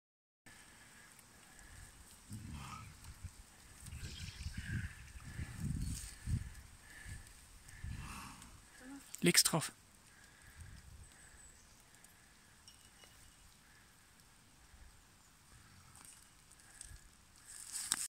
Lynx07 feeding Lynx lynx
Lynx07 konnte durch ruhiges Verhalten und Einfühlungsvermögen diese wundervolle, sichere und seltene Interaktion mit einem außergewöhnlichen Tier durchführen.

Der Eurasische Luchs (Lynx lynx) ist dank vermehrter Schutzbemühnungen und Projekte in den letzten Jahrzehnten wieder heimisch in eingigen Arealen Deutschlands. Der Luchs ist die größte Raubkatze in Europa. Seine Nahrung besteht vorwiegend aus jungem Rehwild (Capreolus capreolus) und Wildkaninchen (Oryctolagus cuniculus). Seine Habitate sind meist bewaldete, beergige Gebiete. Die Sinne Hören und Sehen sind beim Luchs perfekt an seine Überlebensstrategie adaptiert.

Es ist unser Auftrag der nächsten Generation die Flora & Fauna nahe zu bringen um dazu beizutragen diese  auch zukünftig zu erhalten.